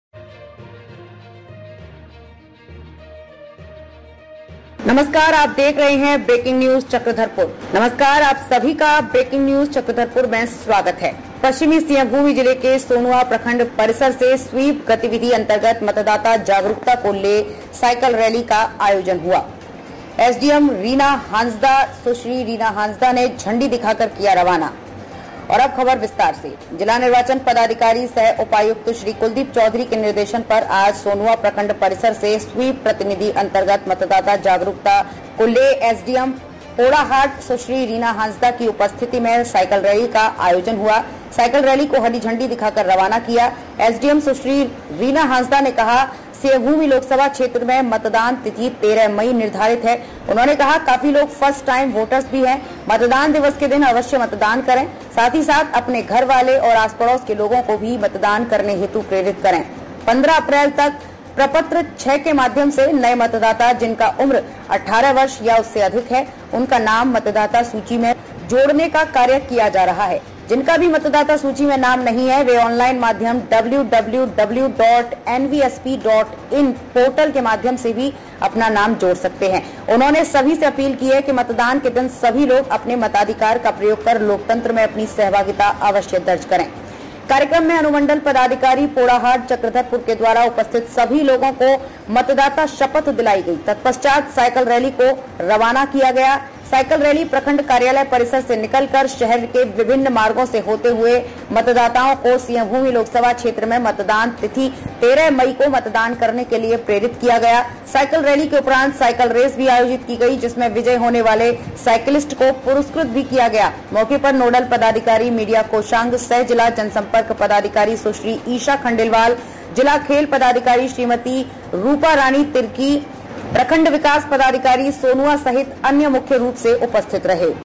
स्वीप गतिविधि अंतर्गत मतदाता जागरूकता के को ले साइकिल रैली का आयोजन हुआ | Breaking News CKP
पश्चिमी सिंहभूम जिले के सोनुआ प्रखंड परिसर से स्वीप गतिविधि अंतर्गत मतदाता जागरूकता के को ले  साइकिल रैली का आयोजन हुआ

एसडीएम रीना हांसदा   सुश्री रीना हंसदा ने झंडी दिखाकर किया रवाना

जिला निर्वाचन पदाधिकारी सह उपायुक्त श्री कुलदीप चौधरी के निर्देशन पर आज सोनुआ प्रखंड परिसर से स्वीप गतिविधि अंतर्गत मतदाता जागरूकता को ले एसडीएम  पोड़ाहाट   सुश्री रीना हंसदा की उपस्थिति में साइकिल रैली का आयोजन हुआ ।साइकिल रैली को हरी झंडी दिखाकर रवाना किया।

एसडीएम सुश्री रीना हंसदा ने कहा सिंहभूम लोकसभा क्षेत्र मे मतदान तिथि 13 मई निर्धारित है। उन्होंने कहा काफी लोग फर्स्ट टाइम वोटर्स भी हैं। मतदान दिवस के दिन अवश्य मतदान करें साथ ही साथ अपने घर वाले! और आस-पड़ोस के लोगों को भी मतदान करने हेतु प्रेरित करें। 15 अप्रैल तक प्रपत्र 6 के माध्यम से नये मतदाता जिनका उम्र 18 वर्ष या उससे अधिक है, उनका नाम मतदाता सूची में जोड़ने का कार्य किया? जा रहा है, जिनका भी मतदाता सूची में नाम नहीं है। वे ऑनलाइन माध्यम पोर्टल के माध्यम से भी नाम जोड़ सकते है। उन्होंने सभी से अपील किया कि मतदान के दिन सभी लोग आपने मताधिकार का प्रयोग कर लोकतंत्र में अपनी सहभागिता अवश्य दर्ज करें।

कार्यक्रम  में अनुमंडल पदाधिकारी पोड़ाहाट- चक्रधरपुर के द्वारा उपस्थित सभी लोगों को मतदाता शपथ दिलाई गई। तत्पश्चात साइकिल रैली को रवाना किया गया साइकिल रैली प्रखंड कार्यालय परिसर से निकल कर शहर के विभिन्न मार्गो से होते हुए मतदाताओं को सिंहभूम लोकसभा क्षेत्र में मतदान तिथि 13 मई को मतदान करने के लिए प्रेरित किया गया। साइकिल रैली के उपरांत साइकिल रेस भी आयोजित किया गया। जिसमें विजय होने वाले साइकिलिस्ट को पुरस्कृत भी किया गया। मौके पर नोडल पदाधिकारी मीडिया कोषांग सह जिला जनसंपर्क पदाधिकारी सुश्री ईशा खंडेलवाल, जिला खेल पदाधिकारी श्रीमती रूपा रानी तिर्की, प्रखंड विकास पदाधिकारी सोनुआ सहित अन्य मुख्य रूप से उपस्थित रहे।